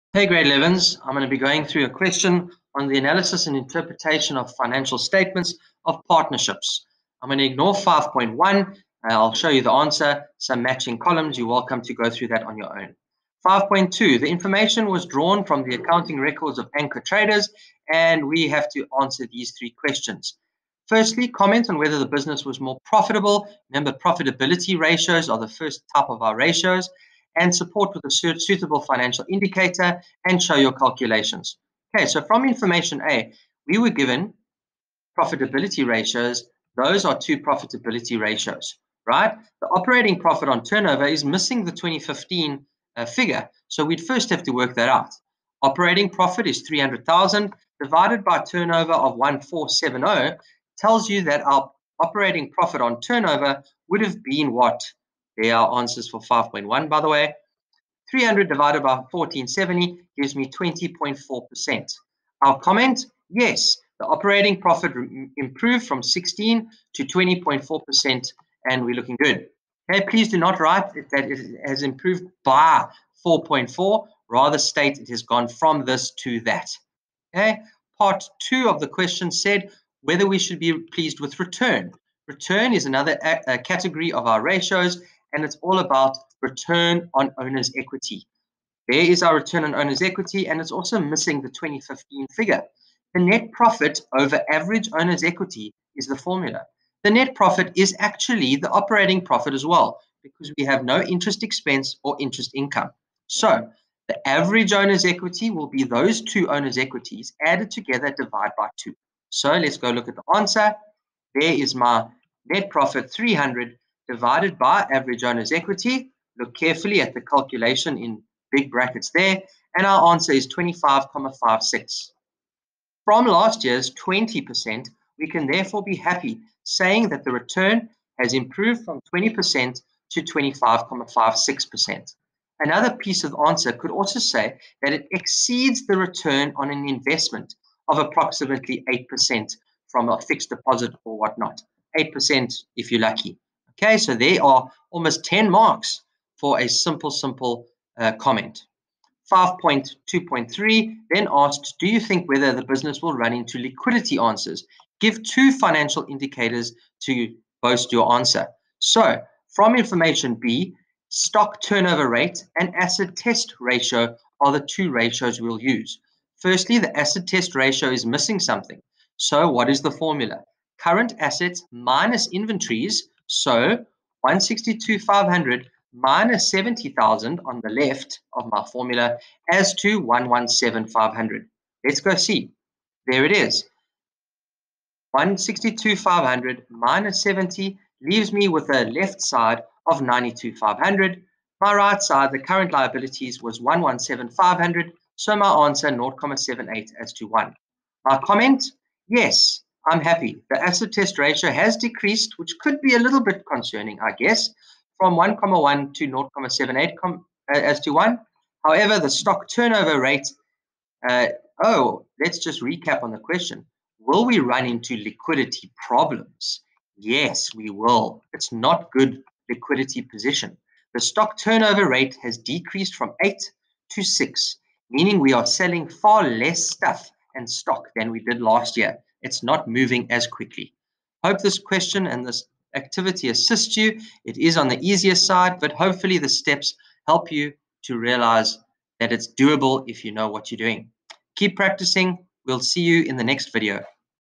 Grade 11 Accounting Ratios and Interpretation
A short task working through some ratios. Look at the memo carefully and learn how to comment. There are easy marks up for grabs if you know what you're doing!